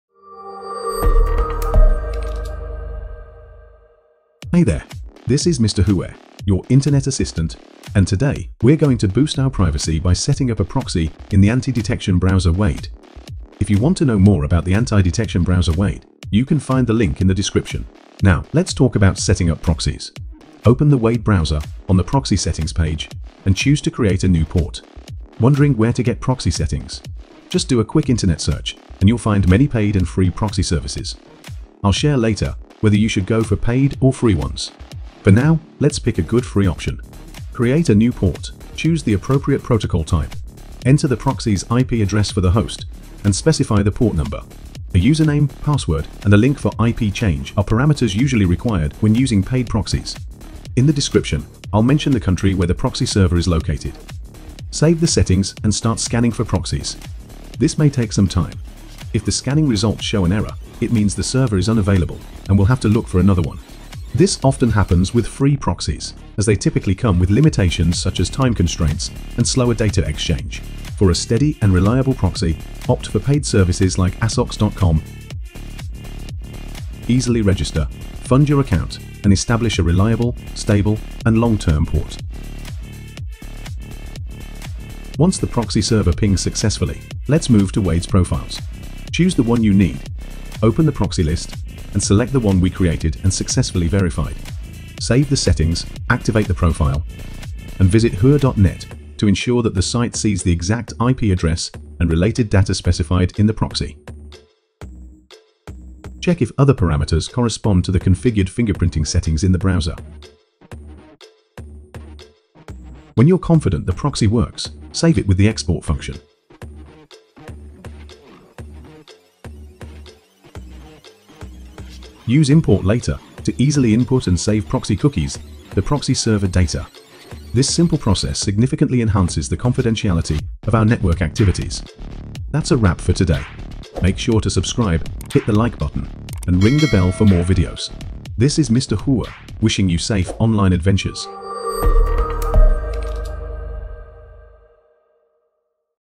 How to configure proxy in anti-detect browser
In this video we'll show you how to set up a proxy in antidetect browser, using WADE Browser as an example.